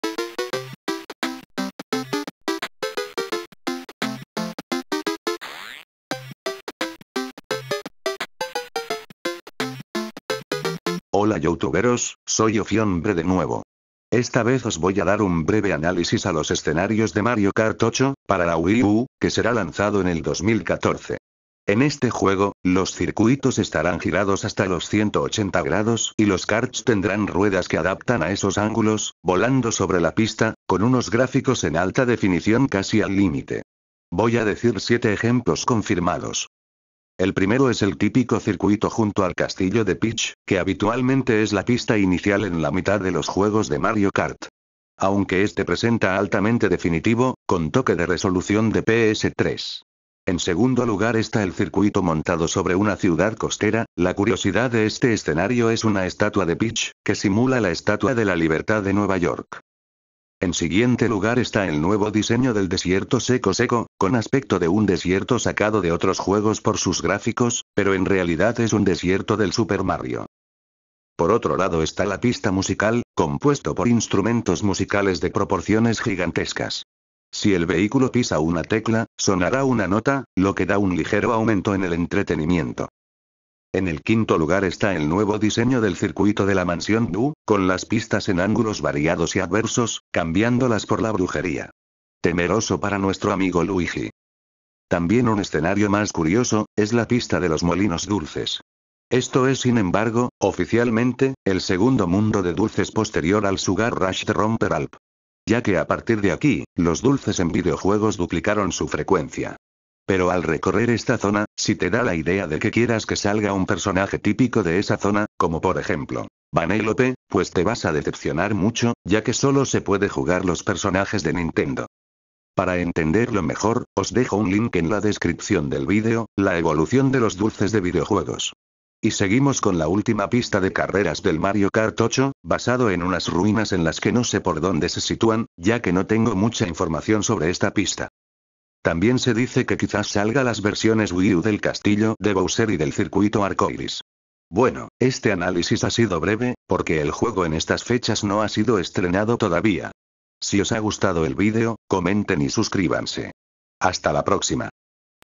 Loquendo- Breve análisis de las pistas del Mario Kart 8 Wii U
Este vídeo es un breve análisis a las pistas del Mario kart de Wii U.